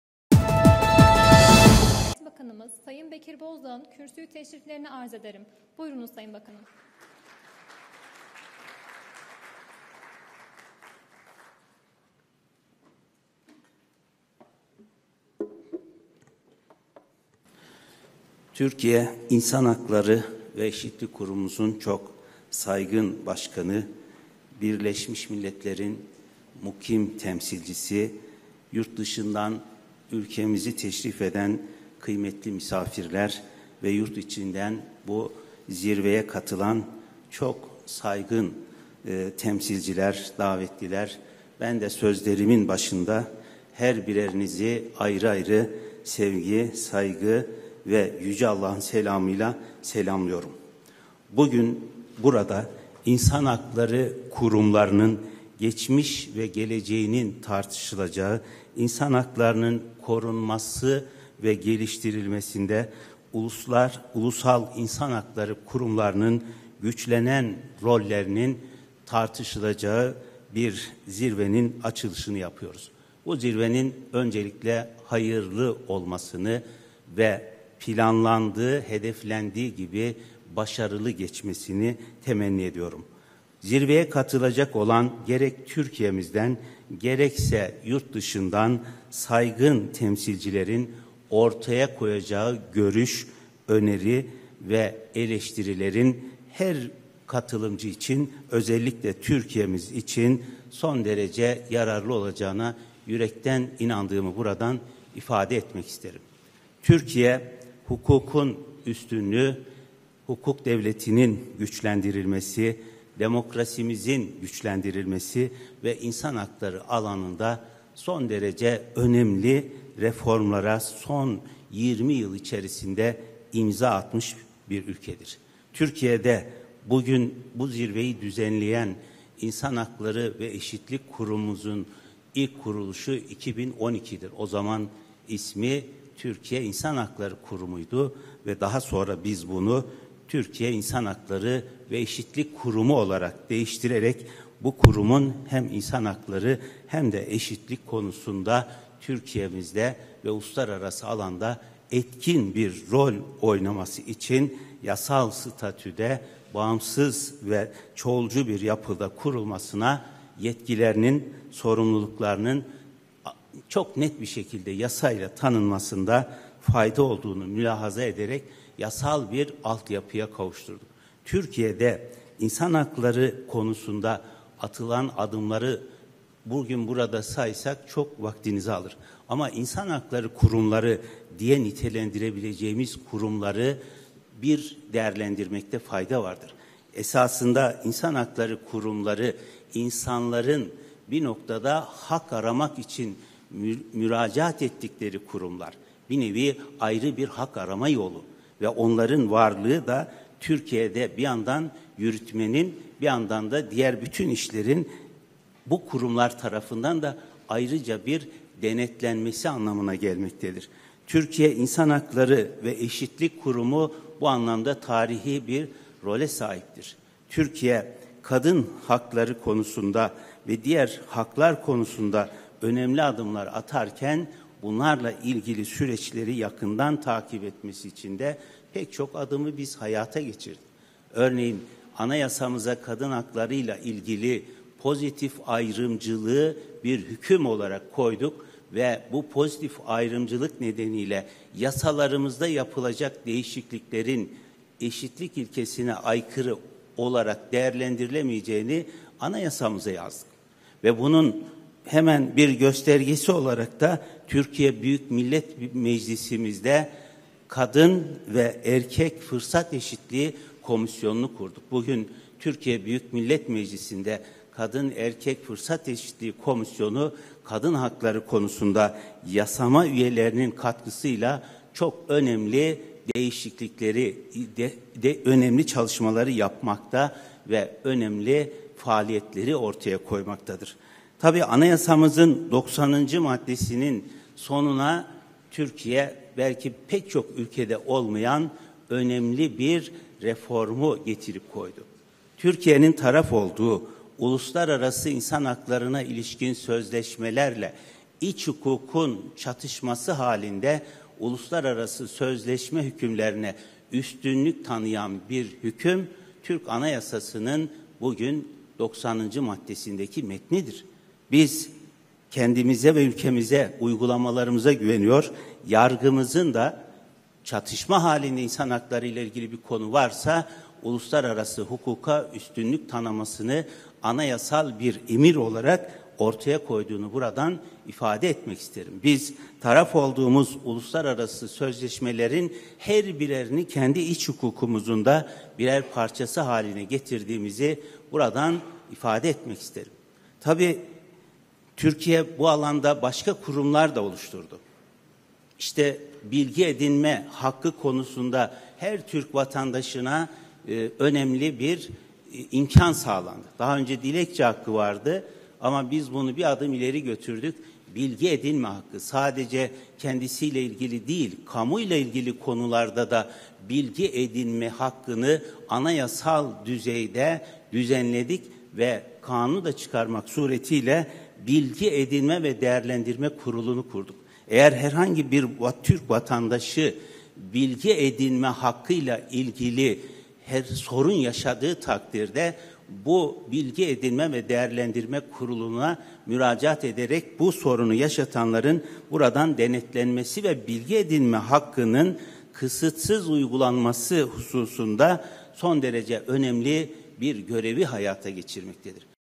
Bakan Bozdağ Anayasamıza Kadın Haklarıyla İlgili Pozitif Ayrımcılığı Bir Hüküm Olarak Koyduk
Adalet Bakanı Bekir Bozdağ, Türkiye İnsan Hakları Ve Eşitlik Kurumu tarafından düzenlenen Uluslararası Zirve’de konuştu